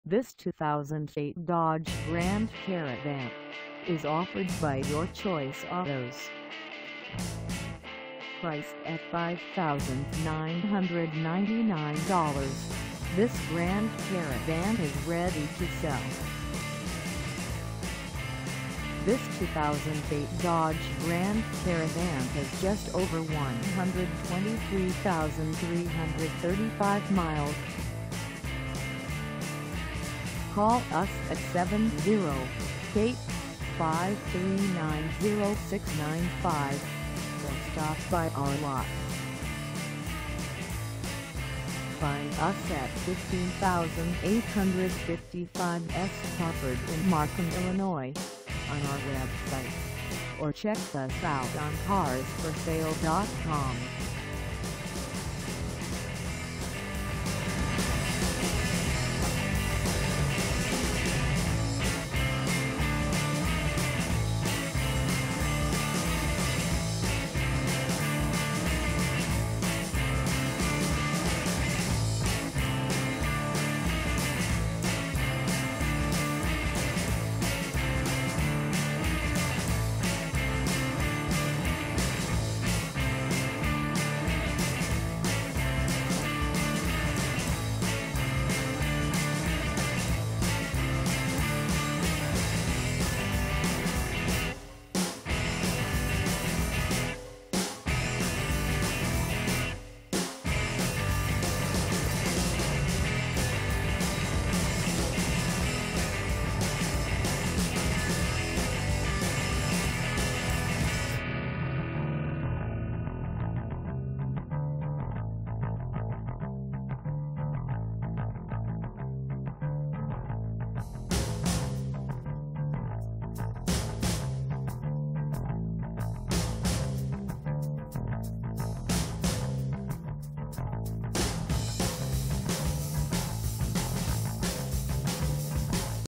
2008 Dodge Grand Caravan SXT Extended 4dr Mini Van for sale
This 2008 Dodge Grand Caravan SXT Extended 4dr Mini Van is for sale in Markham, IL 60428 at Markham.

Engine: 3.8L V6
Transmission: Automatic 6-Speed
Color: Red
Mileage: 123335
Contact Your Choice Autos 15855 S. Crawford Markham, IL 60428 to test drive this 2008 Dodge Grand Caravan SXT Extended 4dr Mini Van today.